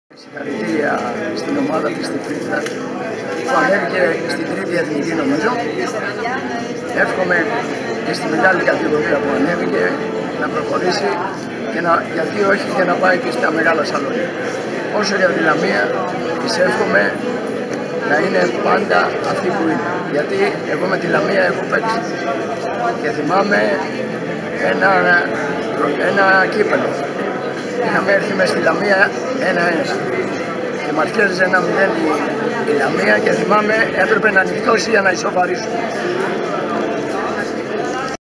Ο θρυλικός Μίμης Δομάζος στη Στυλίδα- Radio NET 101,9
Δείτε περισσότερα εδώ Ο θρυλικός Μίμης Δομάζος στη Στυλίδα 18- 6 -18 τιμητική εκδήλωση σε συνδυασμό με την παρουσίαση του λευκώματος «Μυστικά και…άλλες αποκαλύψεις».